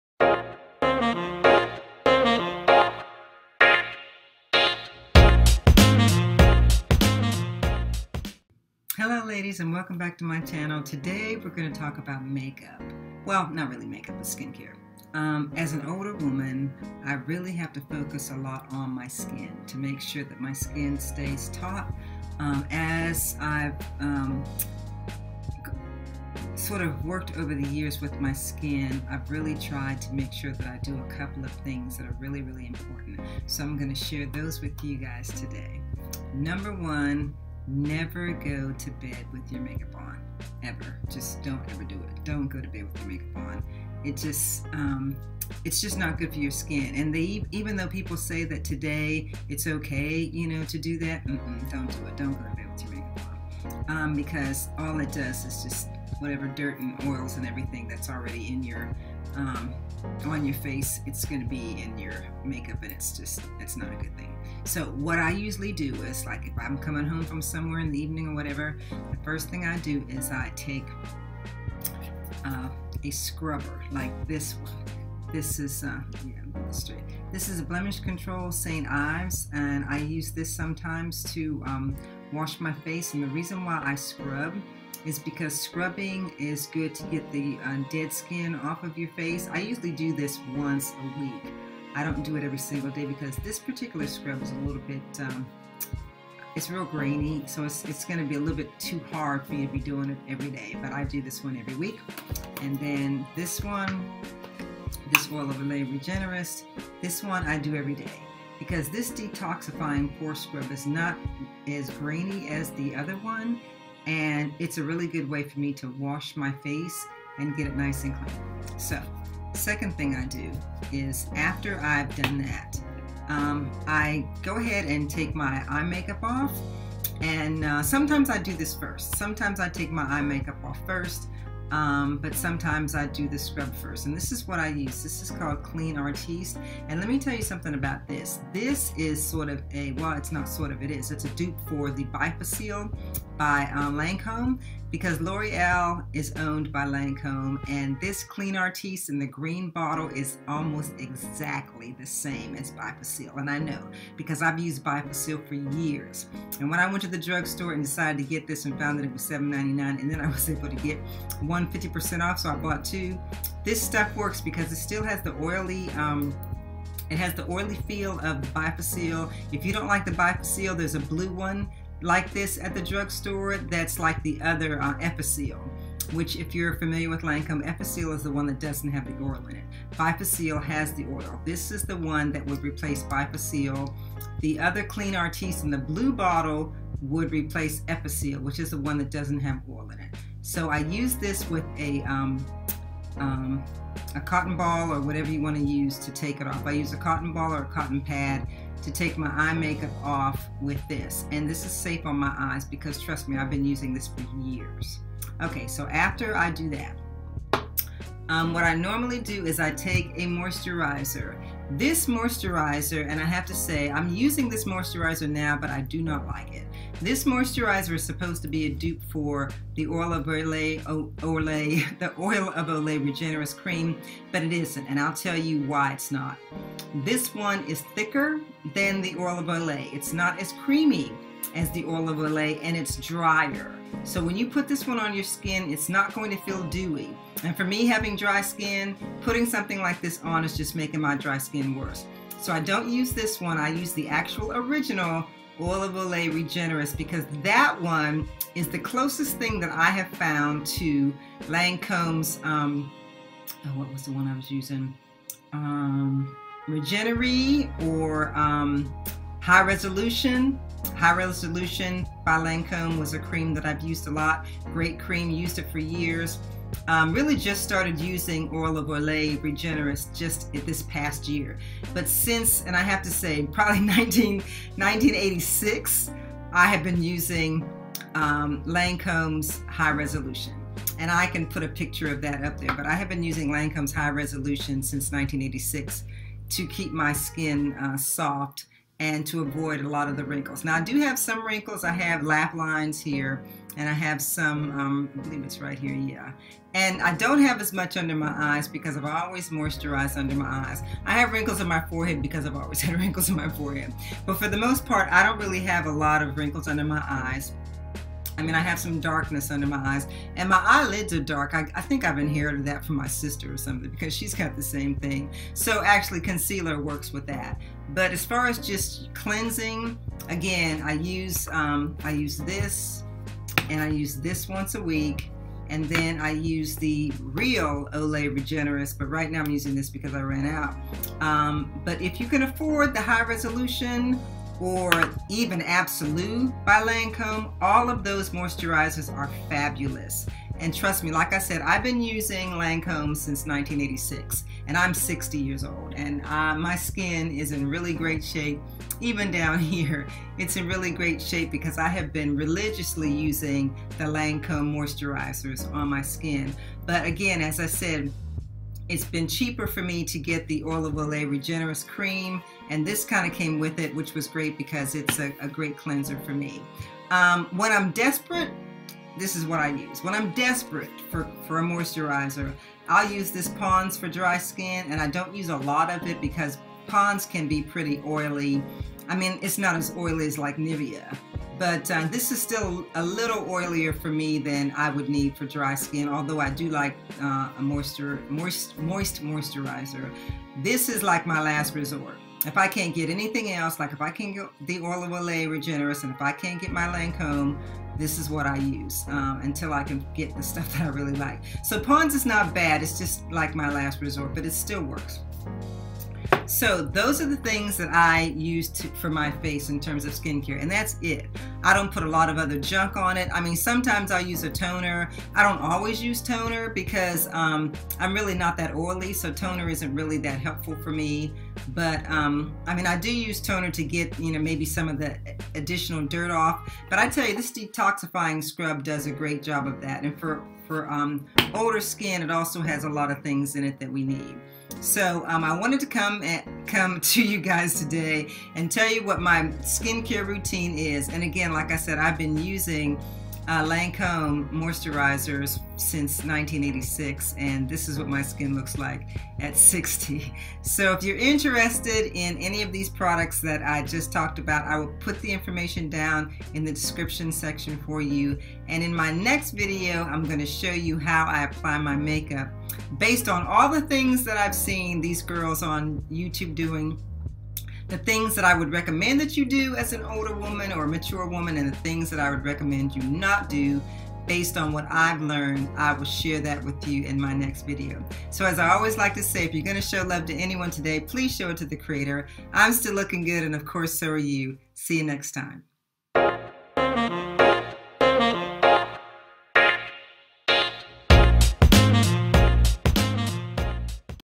HOW I CARE FOR AND CLEAN MY MATURE SKIN
Want to discover a few hacks for some of your favorite Lancome products? Here they are!
Note: My lighting is bright again and yes I am ‘bright skinned’ but not that bright. LOL

//PRODUCTS:
Moisturizers:
Lancome High Resolution - about $80
Olay Regenerist - about $25 (Generic dup is about $14 and can be found at Sally’s Beauty)
Ponds dry skin moisturizer - about $4
Exfoliants:
Olay Regenerist - about $15
St Ives - about $8